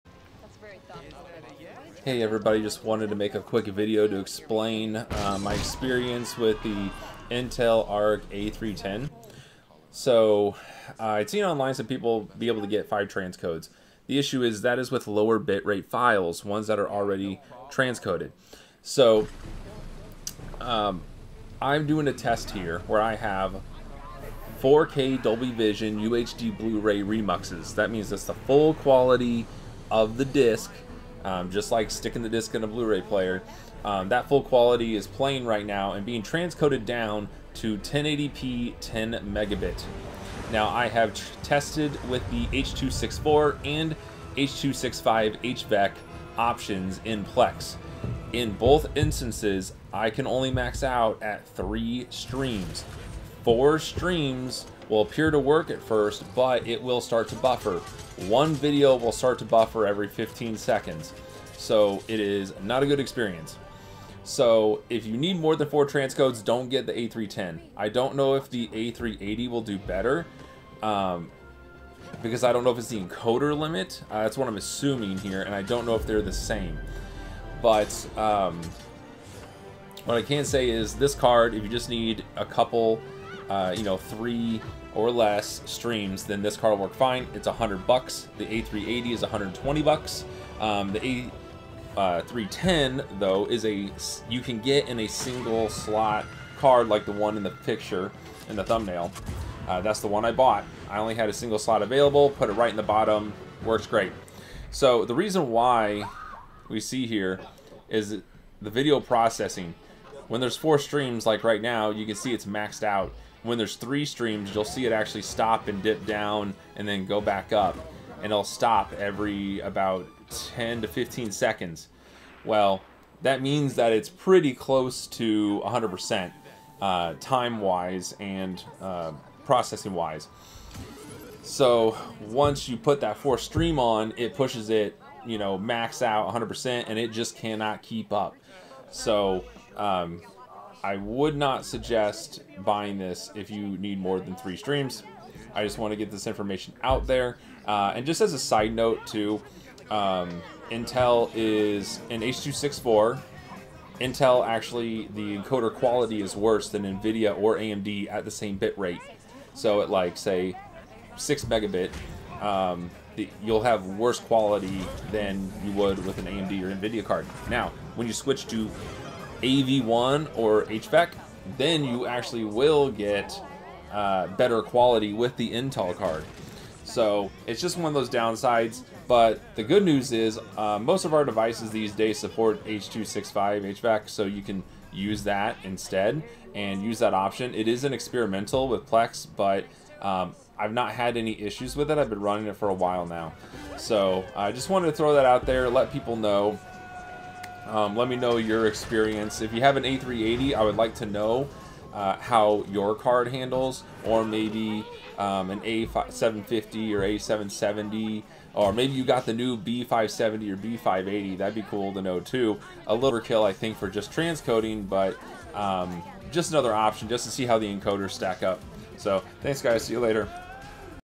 PLEX UHD Bluray Dolby Vision Remux transcoding with the Sparkle Intel Arc 310 ECO
When transcoding 85+Mbps UHD Bluray rips with Dolby Vision down to 1080p 10Mbps, the Intel Arc A310 ECO 4GB can only handle 3 streams, even with a 30 second buffer in PLEX with both X264 and X265 HEVC. With 4 transcodes the Video Processor is at 100% and 1 video will stop to buffer every 15 seconds or so. I've seen other YouTubers get 5 transcodes, but they were using lower bitrate 4K HDR transcoded videos, not remuxes. So you may be able to get more transcodes with lower bitrate videos.